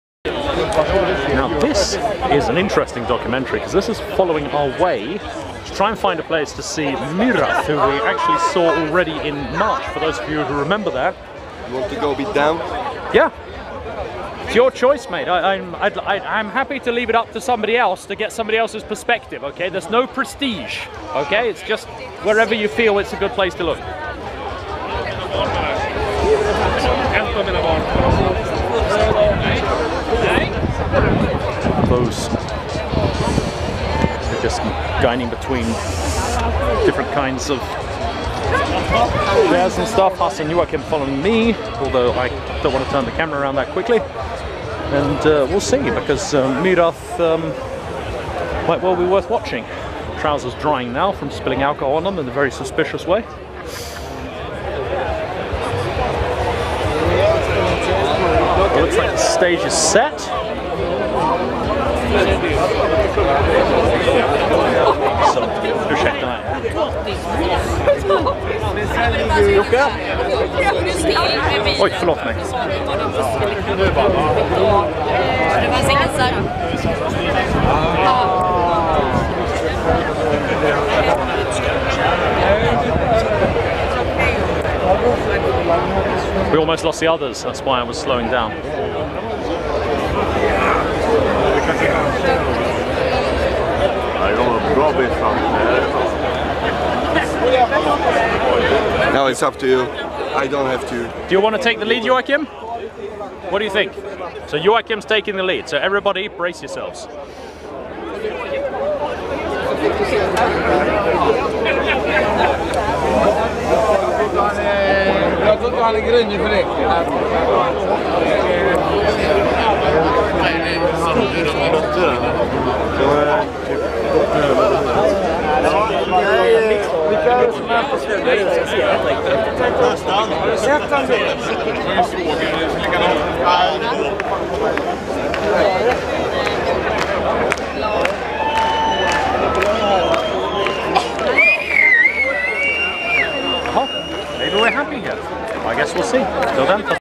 SRF2023 - Video Documentary Part 15 - I guess we're happy here
Thursday at Sweden Rock Festival 2023, me and the lost boys are trying to find a good place to see Myrath from, who are playing on the "Sweden stage"...